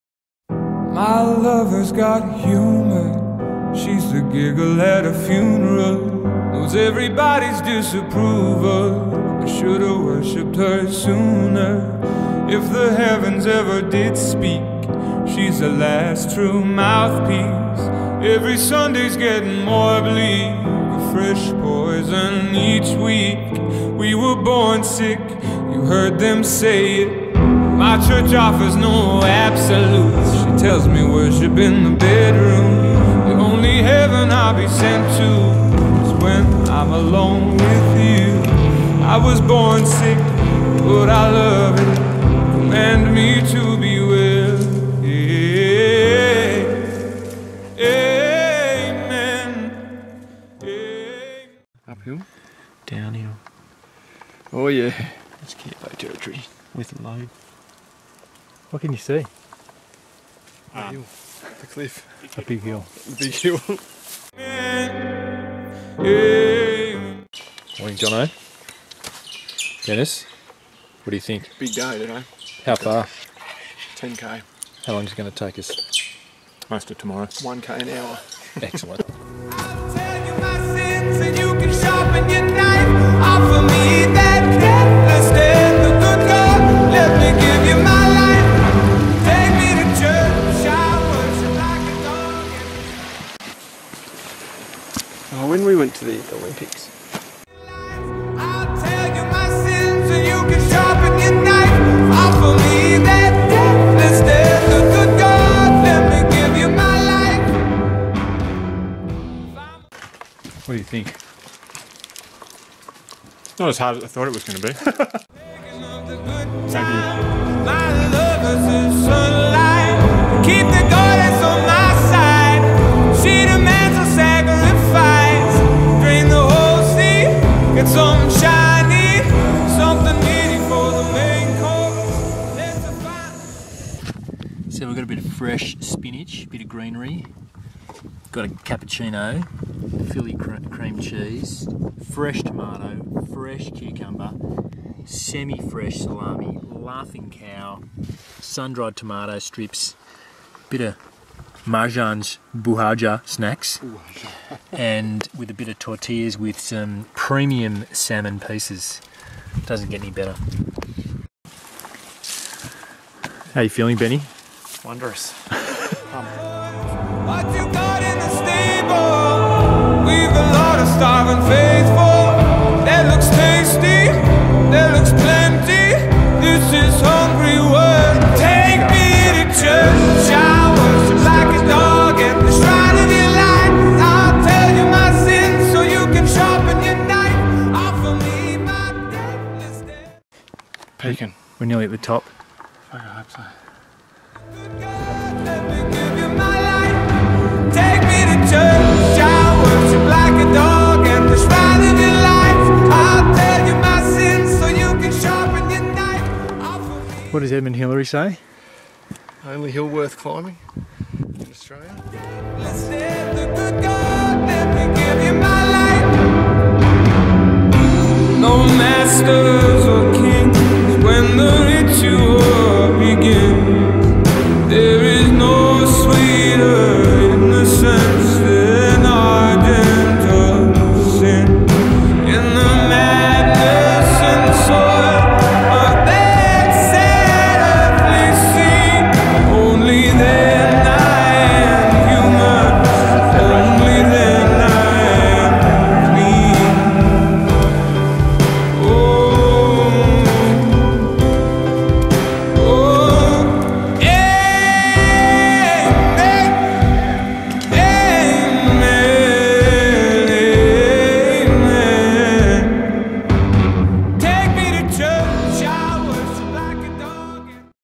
Federation Peak 2017
4 blokes from Canberra. A 40th birthday. 1 giant cliff and lots and lots of scrub.